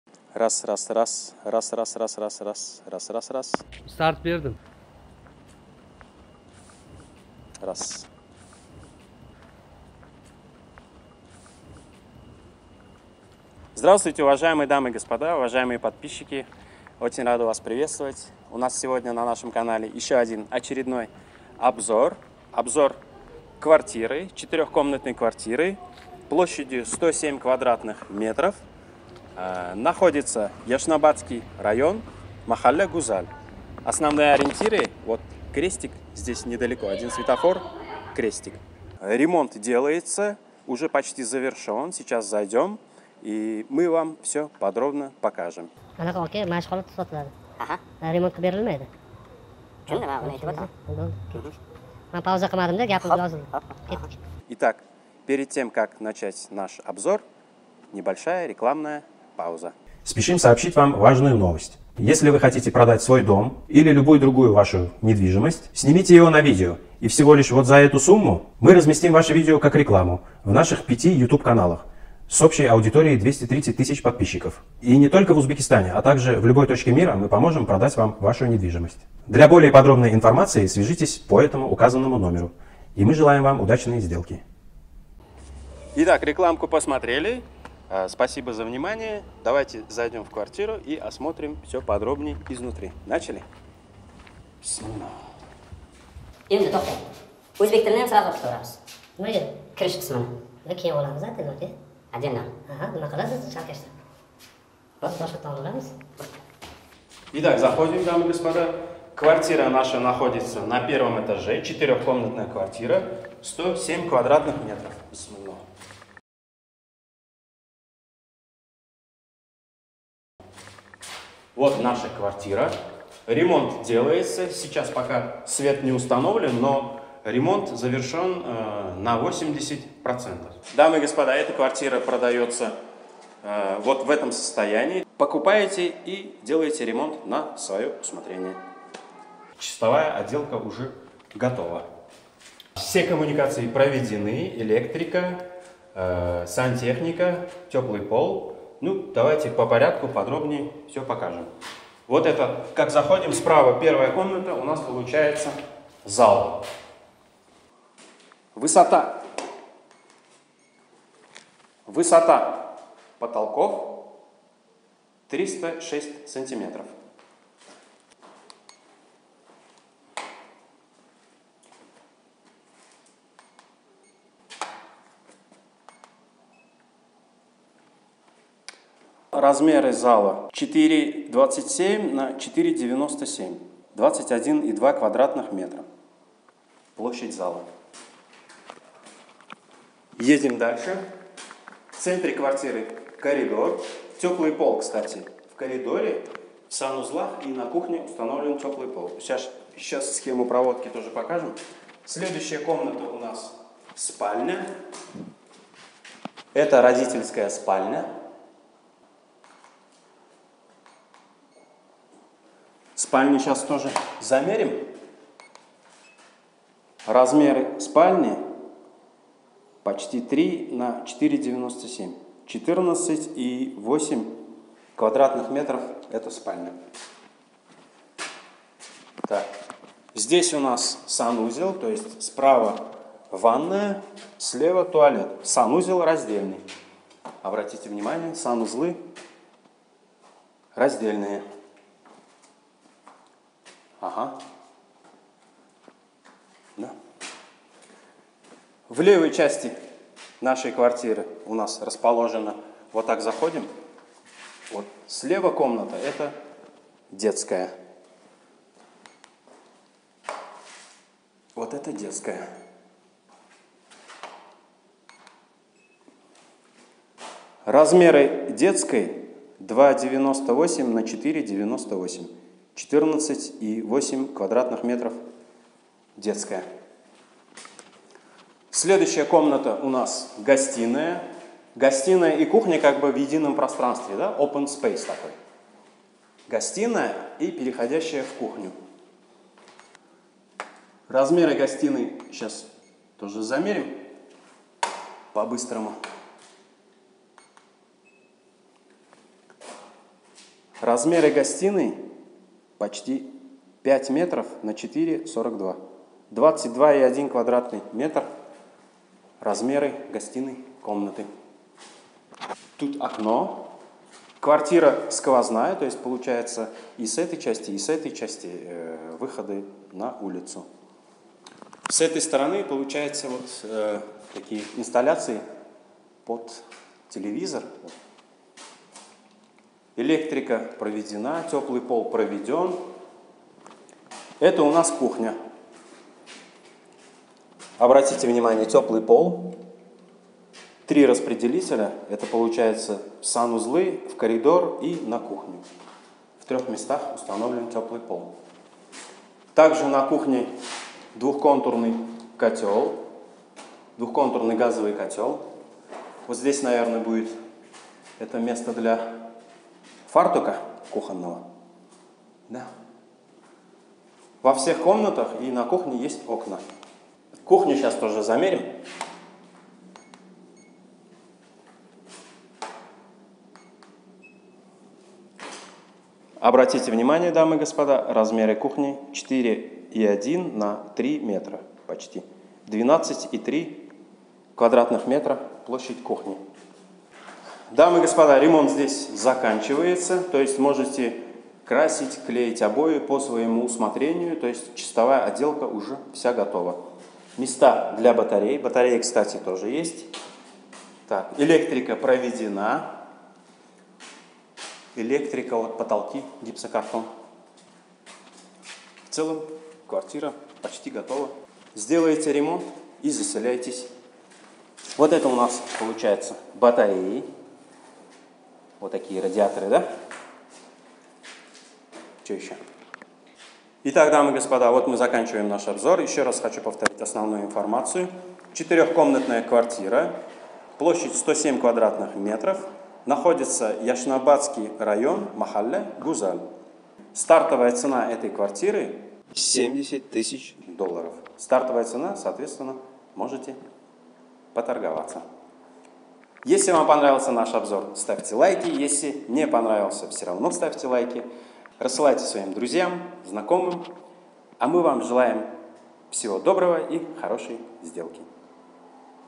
продаётся квартира Ташкент Яшнабад 4 комната 1 этаж цена 70000$
Продается  4 х комнатная квартира на  первом этаже  четырёх этажного дома. Дом новостройка построен 2017 году. Квартира 107 квадратных метров. Адрес Яшнабадские район, ориентир крестик. Ремонт 80% готовый, закончились отделочные работы. Уютная и светлая квартира с удобной планировкой. Свет, газ и вода постоянно. В квартире установлено своё индивидуальное отопление,  теплый пол. Цена: 70.000$ торг уместен. Телефон собственника:
Звоните и уточняйте...
Агарда уй сизга ёқан бўлса яқинларизга юборинг.✅
реклама учун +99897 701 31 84